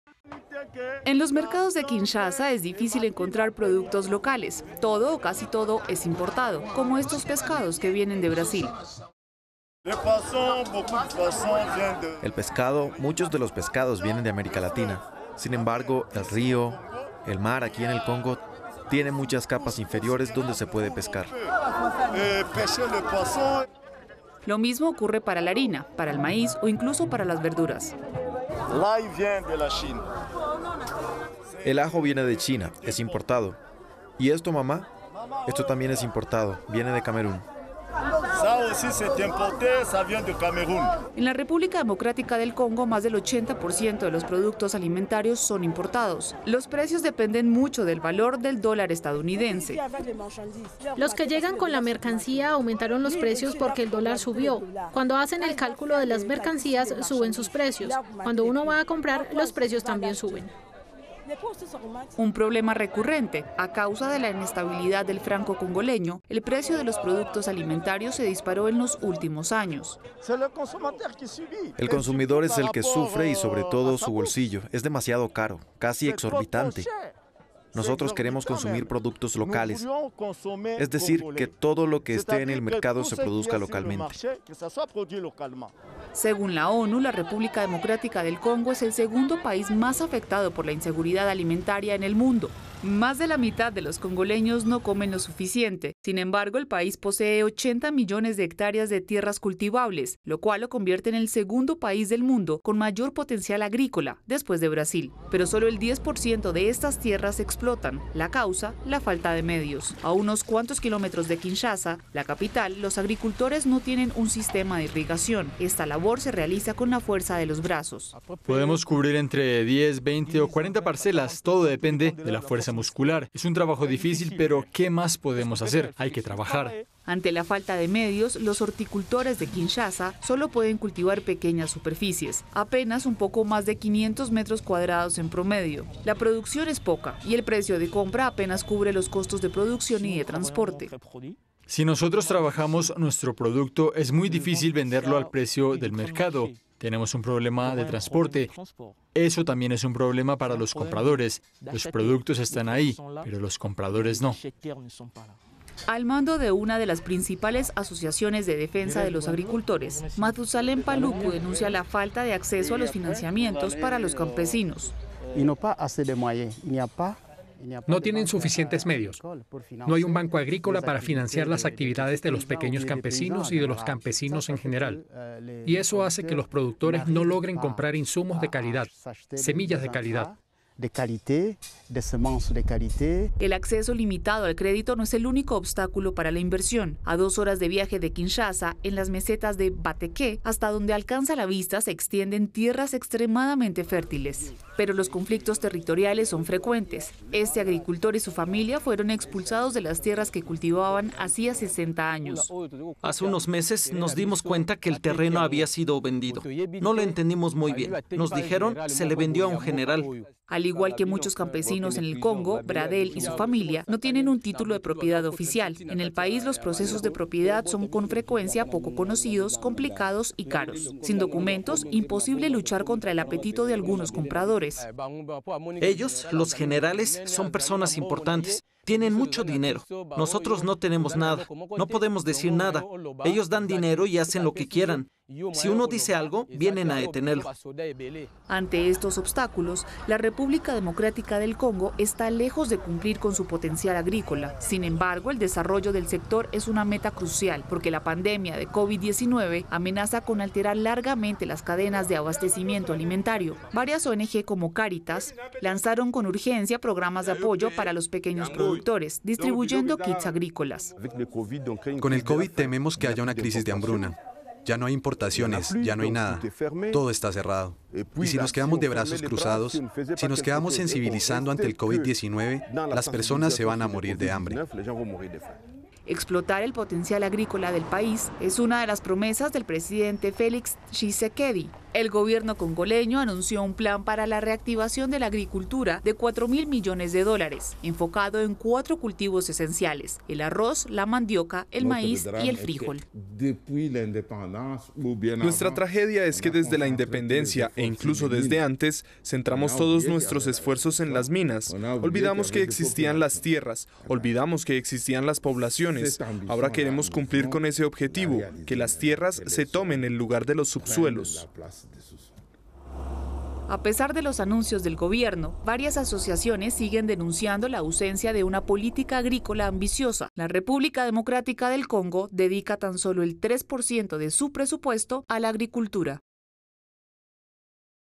Se agrava la inseguridad alimentaria en República Democrática del Congo
República Democrática del Congo es el segundo país del mundo con más tierras cultivables pero solo explota el 10% de esos territorios e importa casi todos sus alimentos. La consecuencia de esta dependencia es la inseguridad alimentaria. En marzo, la pandemia de Covid-19 y el cierre de fronteras agravó la situación. Los precios de los alimentos se dispararon y algunos productos básicos, como la harina de maíz, están agotados. Ahora el Gobierno busca afanosamente medios para desarrollar el potencial agrícola. Reportaje de Reportaje de Juliette Dubois y Clément Bonnerot.